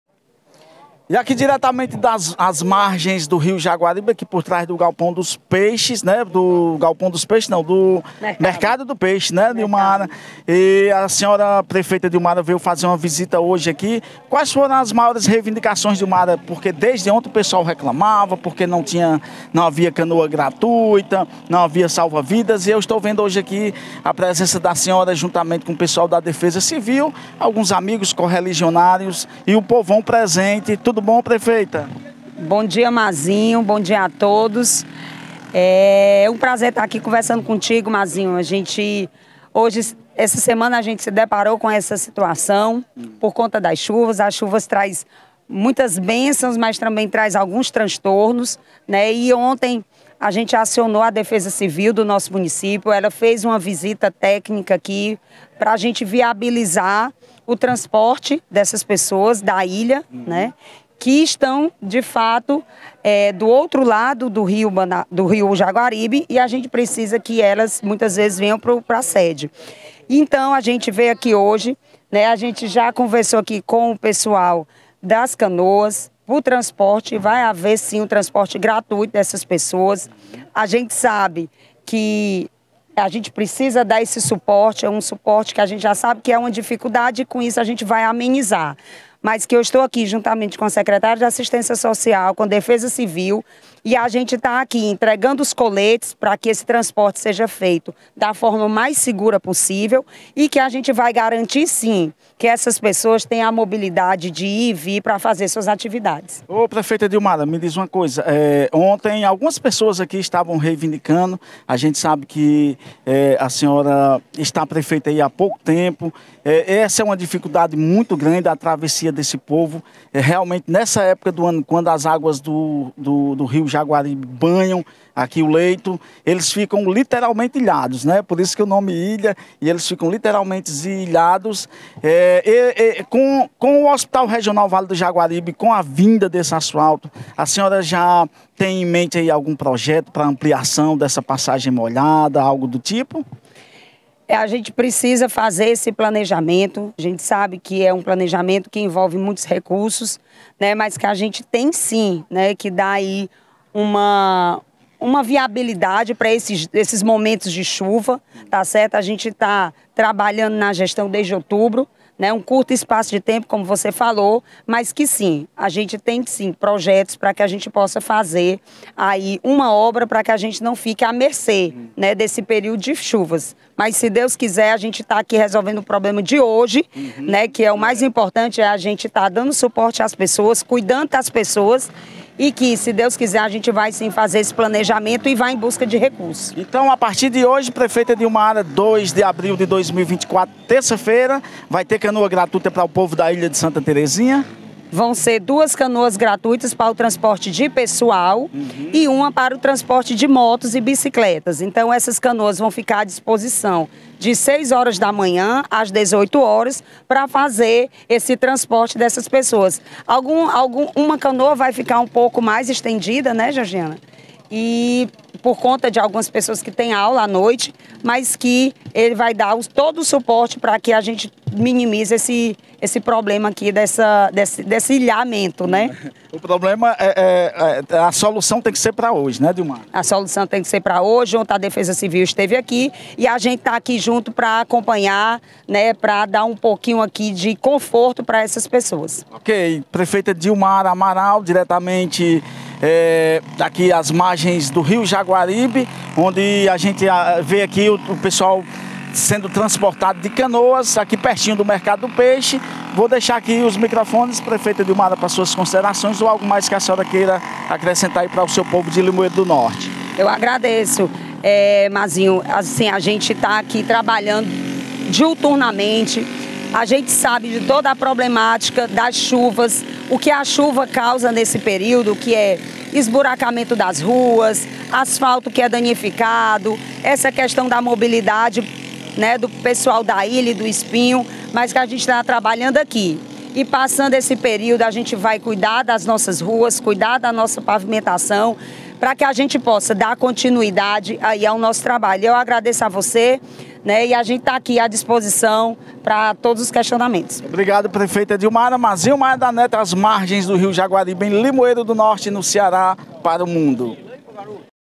Prefeita Dilmara Amaral visita os moradores da Ilha de Santa Terezinha em Limoeiro do Norte-CE.!!
Prefeita Dilmara Amaral faz uma visita aos moradores da comunidade Ilha de Santa Terezinha em Limoeiro do Norte-CE.!!

Na manhã desta terça-feira 02 de abril 2024 acompanhada da defesa civil a prefeita Dilmara Amaral foi ver de perto a situação da enchente das águas as margens do rio Jaguaribe por trás do mercado do peixe no centro da cidade, ela aproveitou a oportunidade e foi ver de perto a real situação dos moradores do bairro Ilha, solucionando o pedido da população para uma canoa gratuita

MAZINHO MAIA DA NET 🇧🇷🌎
"De Limoeiro do Norte-CE para o mundo "